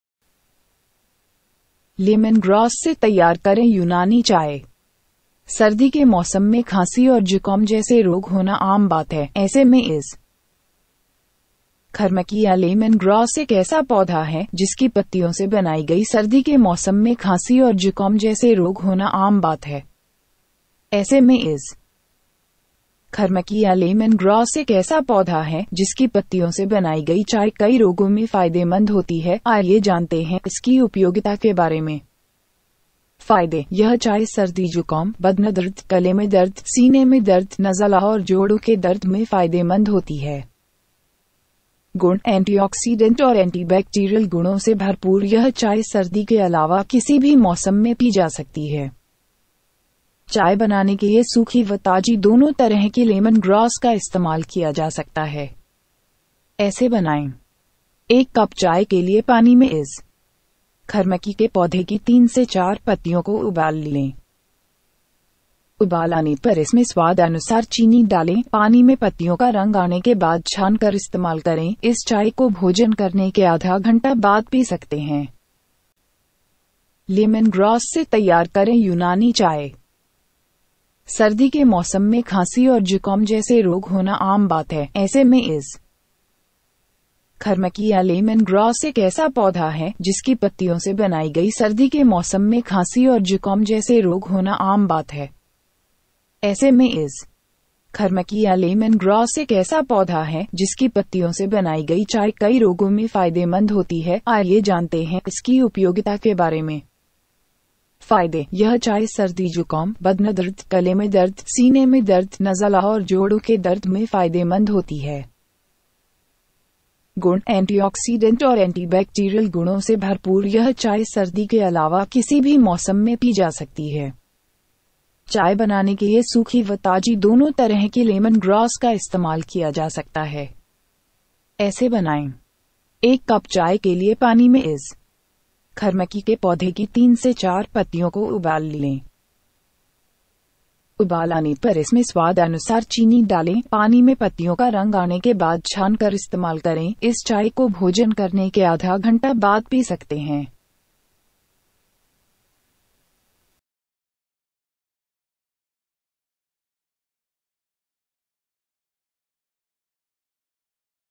Leman Grass Se Taiyar Krein Unani Chai/लेमन ग्रास से तैयार करें यूनानी चाय
Description in English :- Leman Grass Se Taiyar Krein Unani Chai - In winter, it is common to have diseases like cough and cold. In such a situation, Ijkhar Maki or Lemon Grass is such a plant, in which it is common to have diseases like cough and cold in winter season. In such a situation, Ijkhar Maki or Lemon Grass is a plant whose tea made from leaves is beneficial in many diseases. Let's know about its utility.

Benefits: This tea is beneficial in colds, cold, sore throat, chest pains, eyes and joints pain.

Properties: This tea, rich in antioxidant and antibacterial properties, can be drunk in any season except winter. Both dry and fresh lemon grass can be used to make tea.

Create like -
Boil 3-4 leaves of Ejkhar Maki plant in water for a cup of tea. After boiling, add sugar as per taste. After the color of the leaves in the water, filter and use. You can drink this tea half an hour after having your meal.